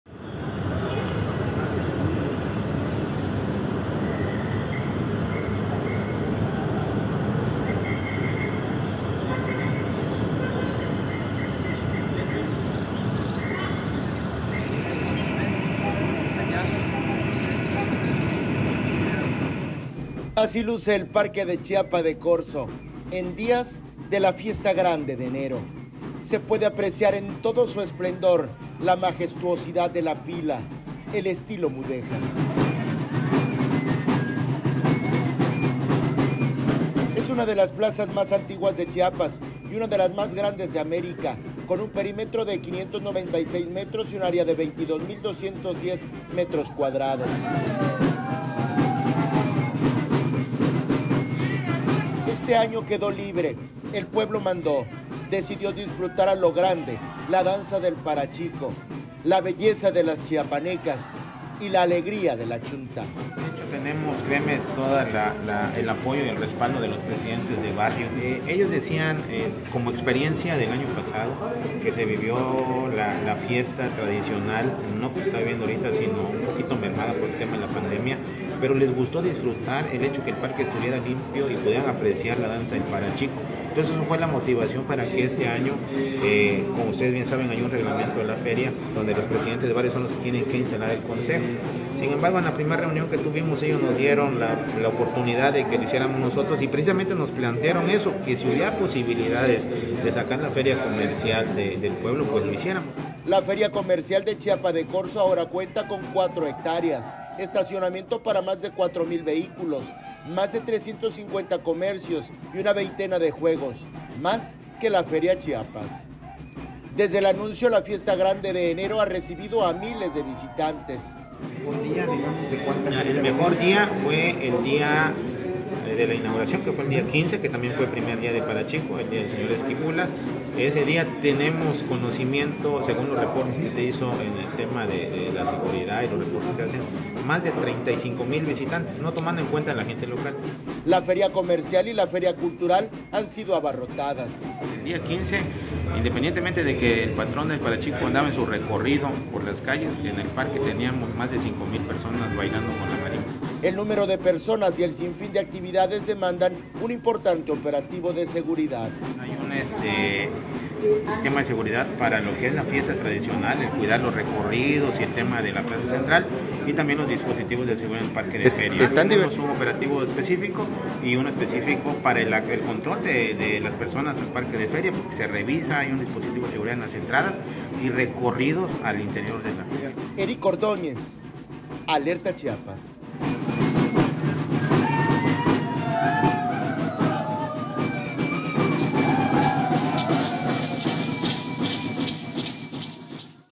En Chiapa de Corzo el pueblo mandó a mover la feria
La plaza considerada de las más antiguas de Chiapas y una de las más grandes de América, quedó libre para la danza del parachico, la belleza de las chiapanecas y la algarabía de la chuntá.